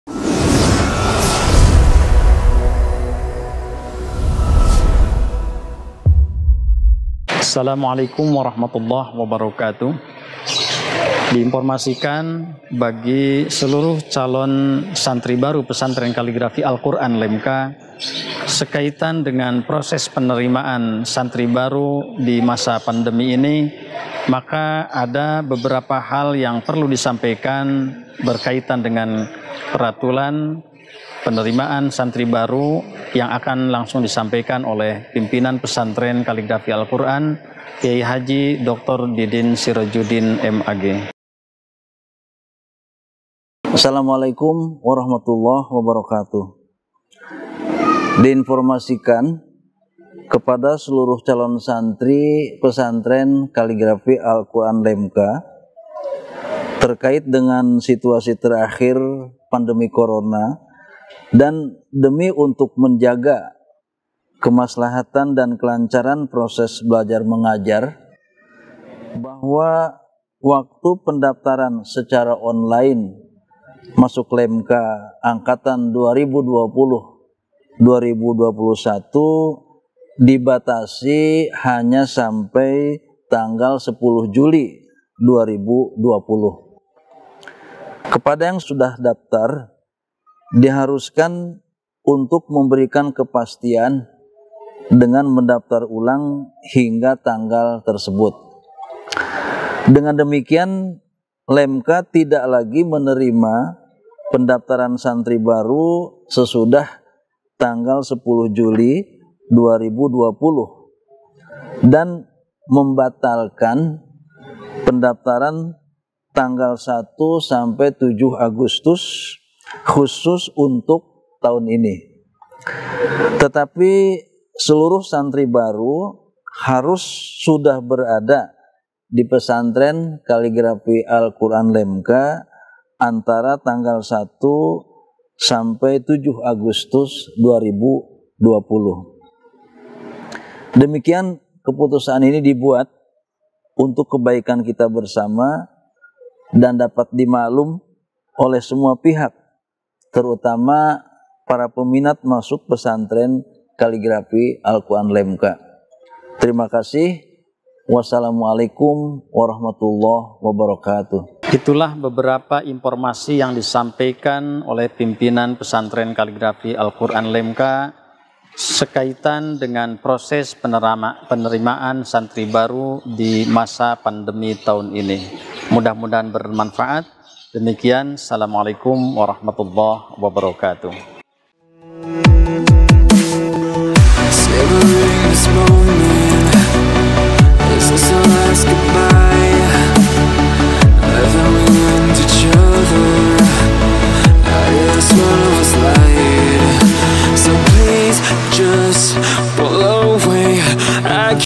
CALON SANTRI LEMKA WAJIB NONTON MAKLUMAT DARI PIMPINAN PESANTREN UNTUK PENDAFTARAN SANTRI BARU 20/21
Di informasikan kpd. seluruh calon santri pesantren kaligrafi alquran lemka, terkait dengan situasi terakhir pendemi corona dan demi untuk menjaga kemaslahatan dan kelancaan proses belajar mengajar.
      bahwa waktu pendaftaran secara online lemka angkatan 2020/2021 dibatasi hanya sampai tanggal 10 juli 2020 kepada siapa pun yang sudah daftar diharuskan untuk memberikan kepastian dengan mendaftar ulang hingga tanggal tersebut
   dengan demikian lemka tidak lagi menerima pendaftaran santri baru sesudah tanggal 10 juli 2020 dan membatalkan pendaftaran tanggal 1-7 agustus khusus tahun ini.tetepi santri baru harus sudah berada di lemka antara tangal 1 s/d 7 agustus 2020.
      demikian keputusan ini dibuat untuk kebaikan kita bersama dan agar dimaklumi oleh semua pihak terutama para peminat masuk pesantren kaligrafi alquran lemka wallahul musta'an .terima kasih .wassalam.
Dr.KH.Didin Sirajudin AR.M.Ag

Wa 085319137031

Follow kami genks di instagram
@LEMKA MEDIA CENTRE

@Lemka Lembaga kaligrafi Alquran

fanspage
@Pesantren Kaligrafi Alquran Lemka Kota Sukabumi